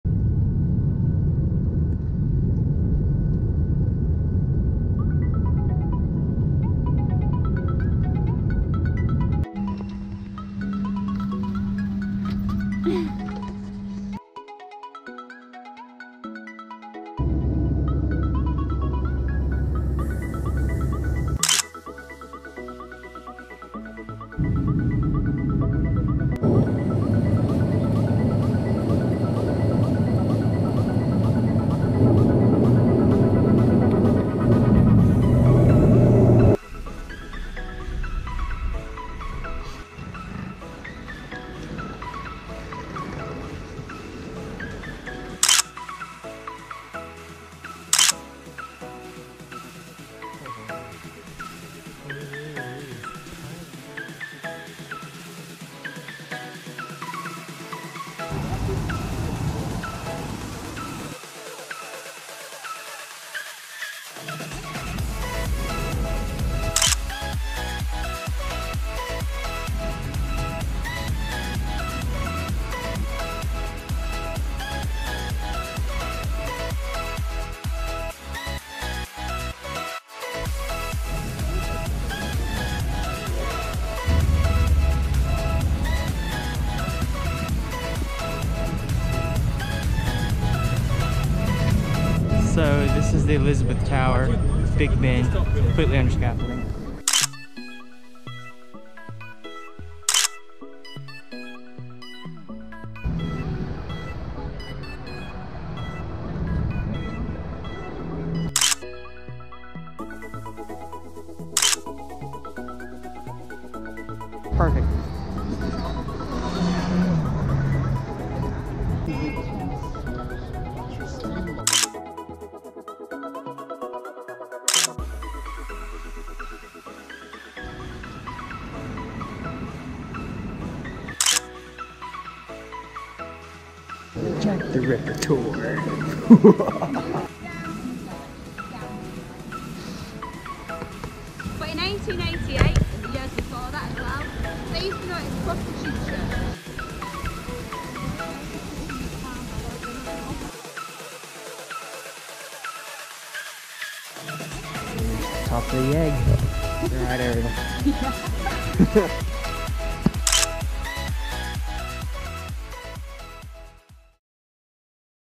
Our First Time in London!
Walking the Thames and going on a Jack the Ripper Tour!
UK Trip 2019: Day 1